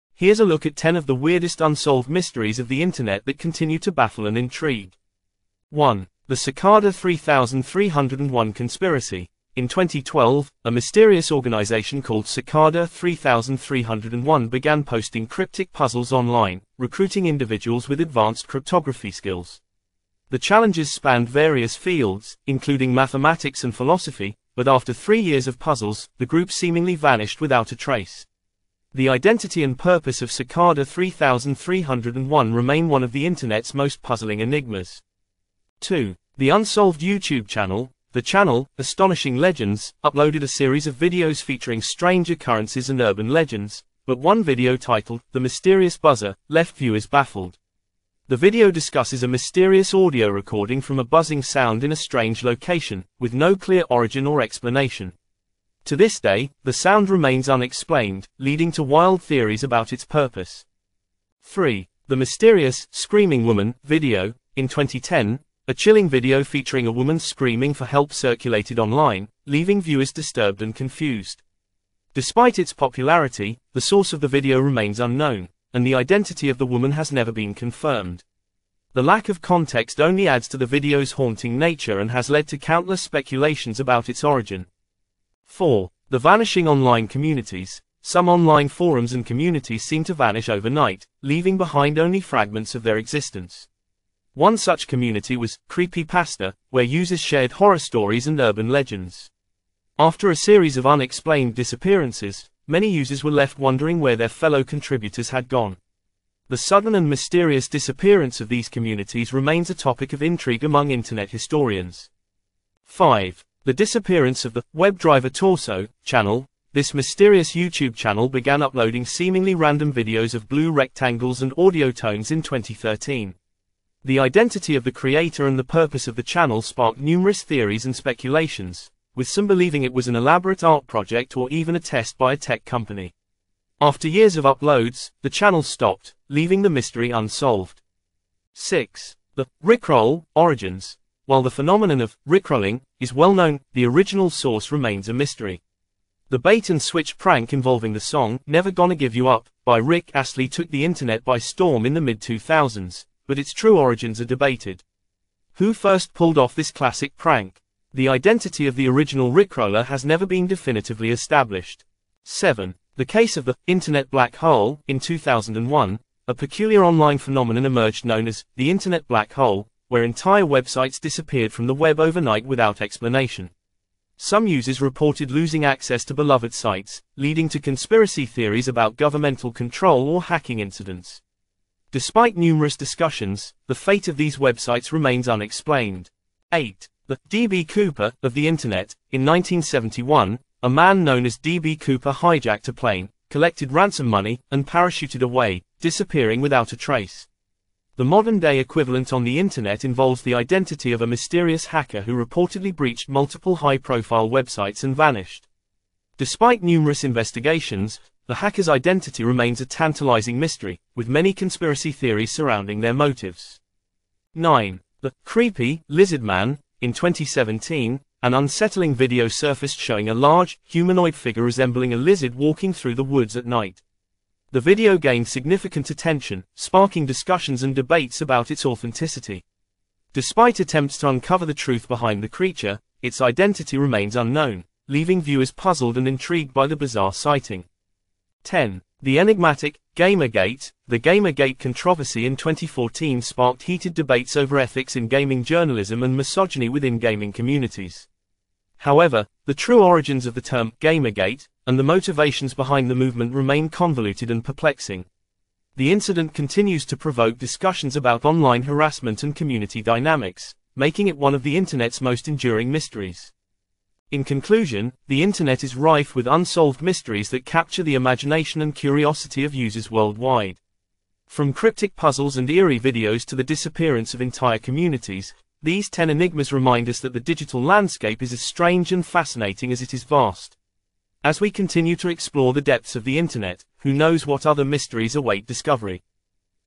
Top 10 Weirdest Unsolved Mysteries of the Internet
The internet is full of mysteries! Explore the top 10 weirdest unsolved mysteries online that continue to baffle and intrigue people around the world.